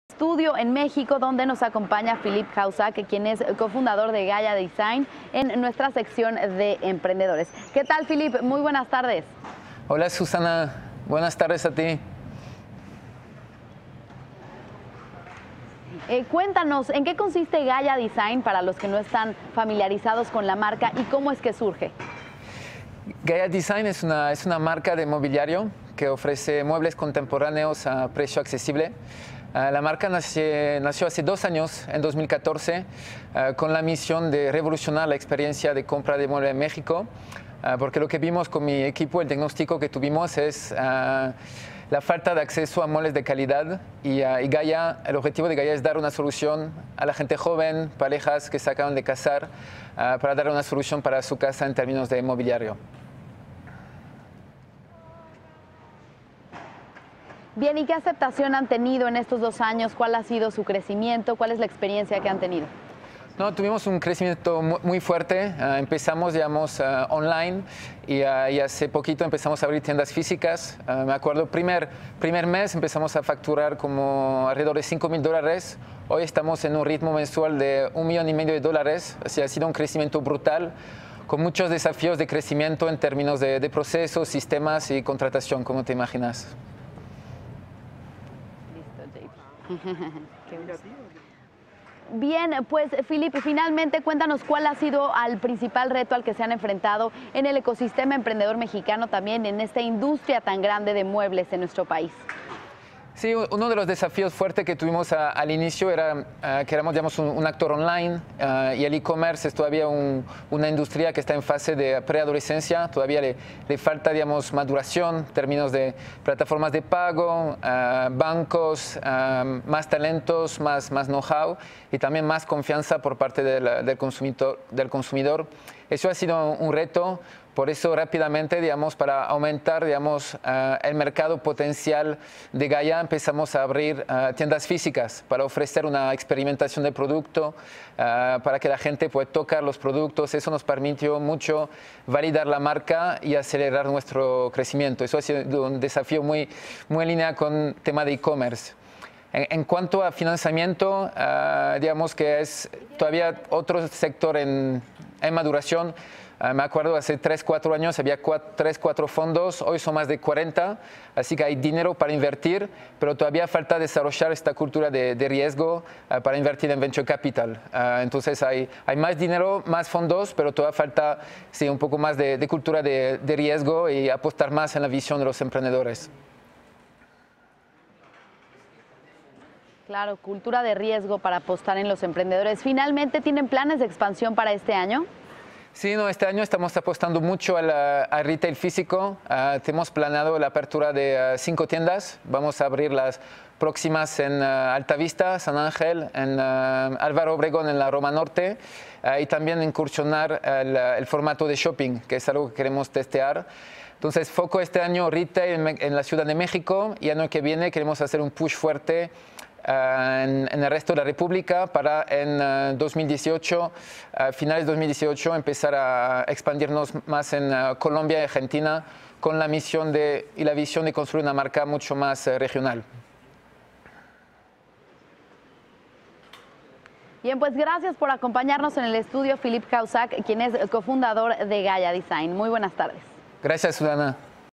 Gaia Design, una marca de muebles contemporáneos a precios accesibles
Philippe Cahuzac, co founder de Gaia Design. Tema: Emprendedor.